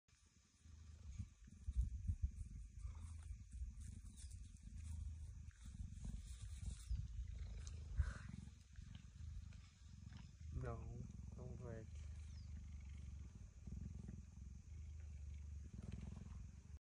Photographer gets up close with cheetah
In this video, Casey M. a wildlife photographer traveled to South Africa to photograph a cheetah. The cheetah slowly approaches him and asks for cuddles!